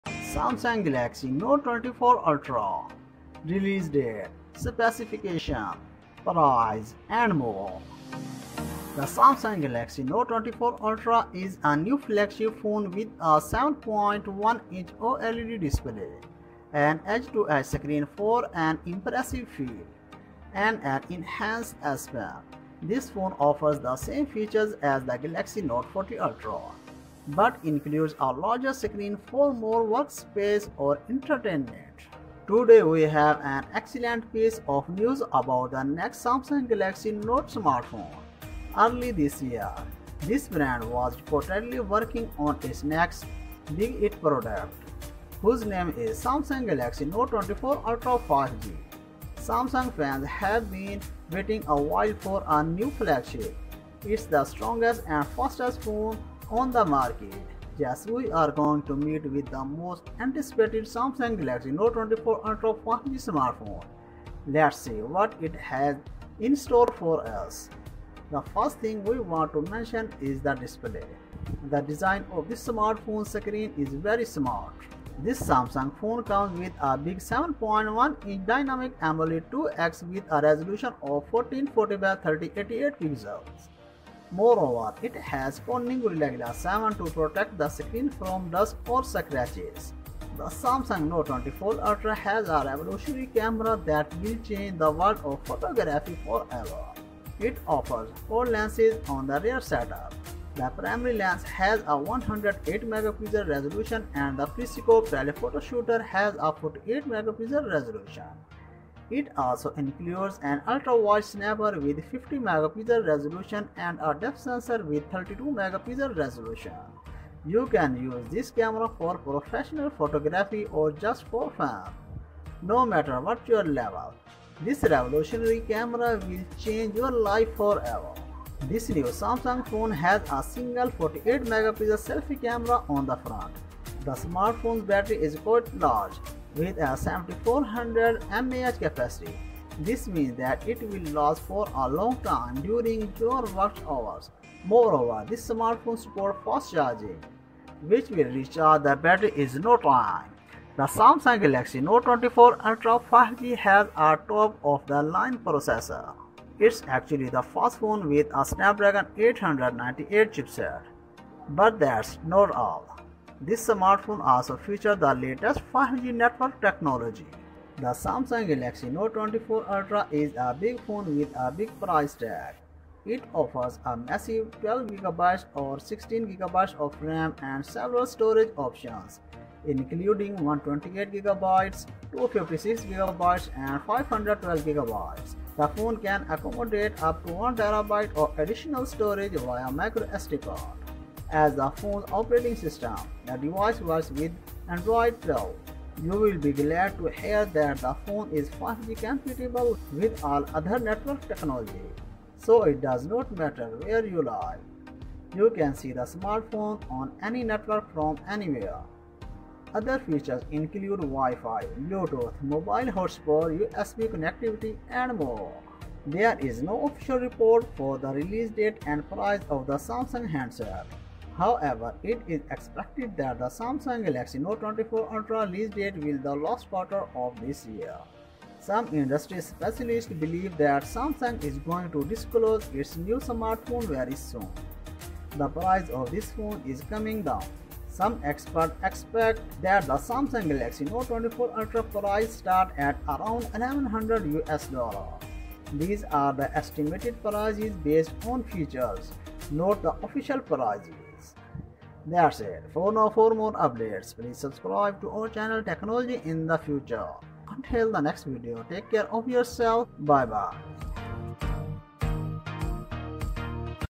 Samsung Galaxy Note 24 Ultra - 5G, Price & Release Date, Specs, First Look, Trailer, Concept!
Samsung Galaxy Note 24 Ultra Release Date, Specs, Price. This Samsung phone comes with a big 7.1-inch Dynamic AMOLED 2X with a resolution of 1440 x 3088 pixels. Moreover, it has Corning Gorilla Glass 7 to protect the screen from dust or scratches. The Samsung Note 24 Ultra has a revolutionary camera that will change the world of photography forever. It offers quad lenses on the rear setup. The primary lens has a 108MP resolution and the periscope telephoto shooter has a 48MP resolution. It also includes an ultra-wide snapper with 50MP resolution and a depth sensor with 32MP resolution.